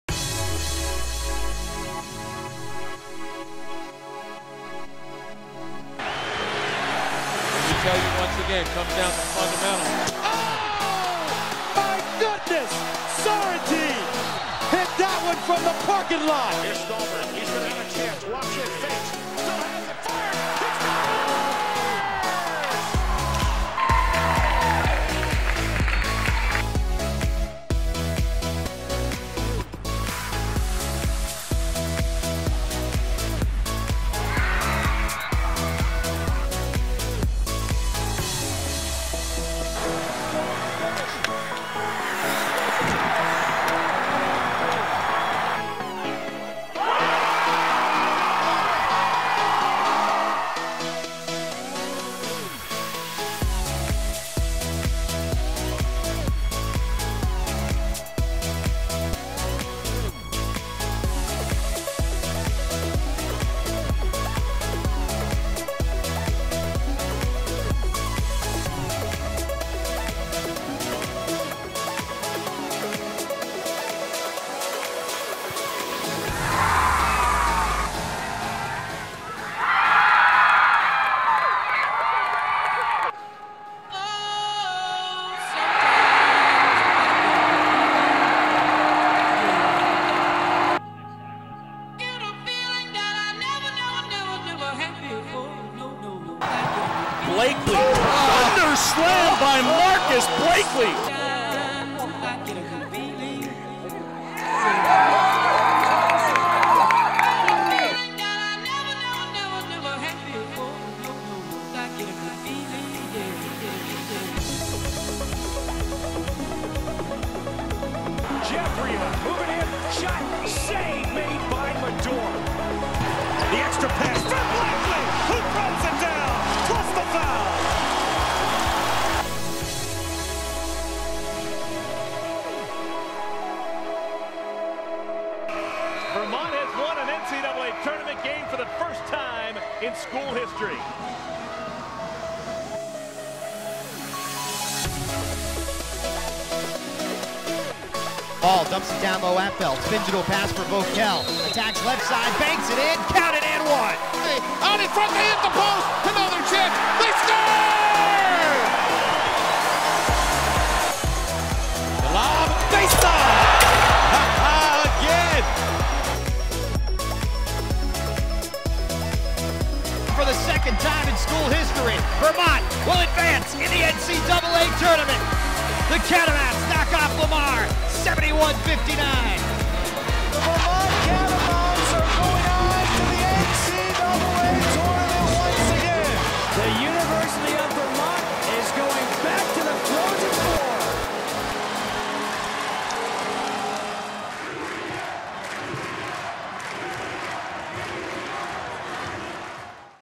Vermont Athletics - Victory Club 10th Anniversary Video
This Saturday the department celebrated the Victory Club's 10th anniversary, take a look back at what we have achieved and built thanks to your support.